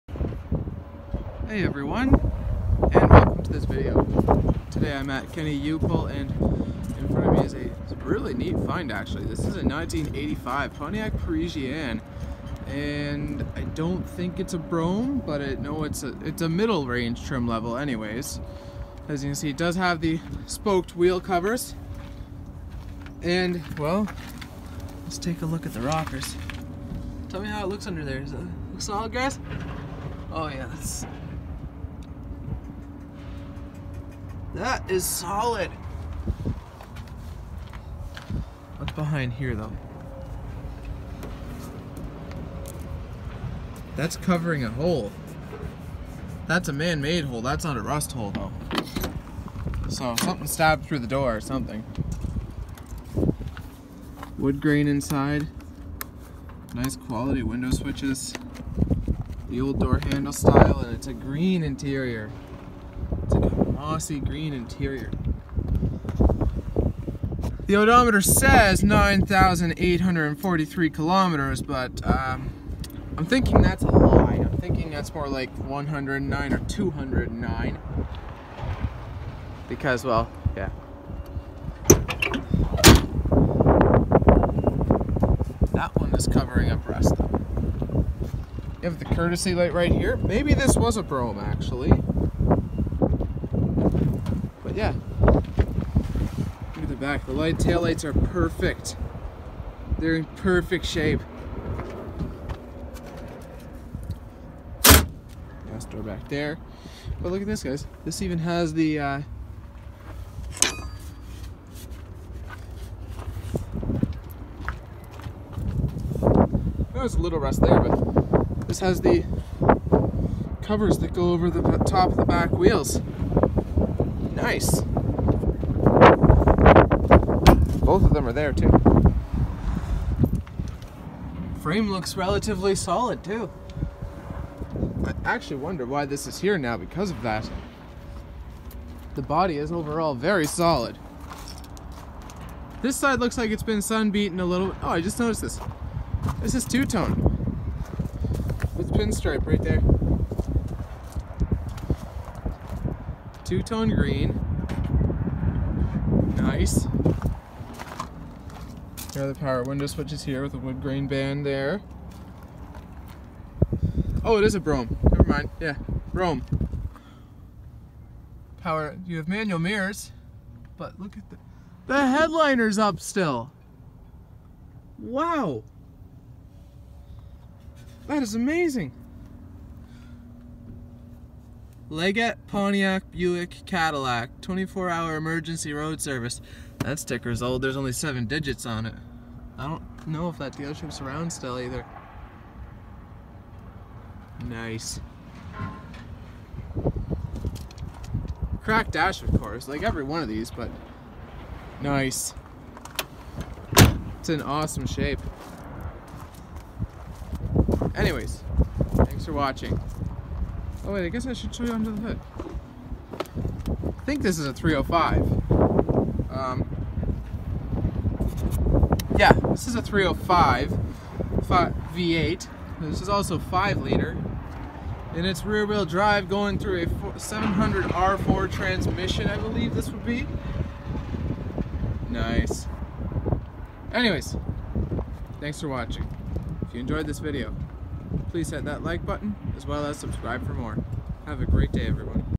Classic Two Tone Green Pontiac Parisienne at the Junk Yard
In this video i will be showing you an old Pontiac Parisienne at the junk yard. It is in quite decent condition and it is two tone green body.